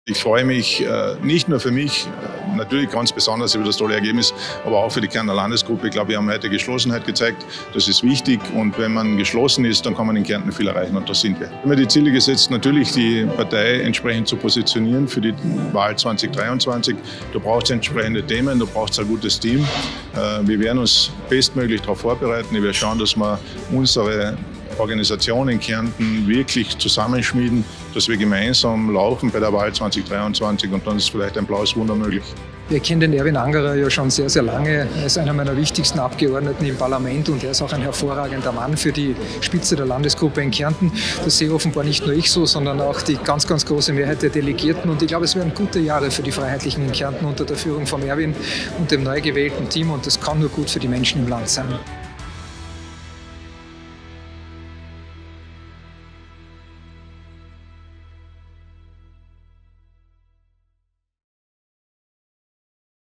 Die schönsten Eindrücke vom Landesparteitag 2021 der FPÖ-Kärnten!
Der gf. Landesparteiobmann der FPÖ-Kärnten, Erwin Angerer, wurde am Samstag beim 32. Ordentlichen Landesparteitag auf der Burg Taggenbrunn mit unglaublichen 95,75 % der Stimmen als Landesparteiobmann bestätigt. Erwin ist ein Garant für Geradlinigkeit, Ehrlichkeit und Bodenständigkeit. Die Kärntner wissen, dass sie sich in allen Situationen auf ihn verlassen können. 🤝🏻
FPÖ-TV hat für Euch die besten und schönsten Eindrücke vom Kärntner Landesparteitag 2021 gesammelt. 🇦🇹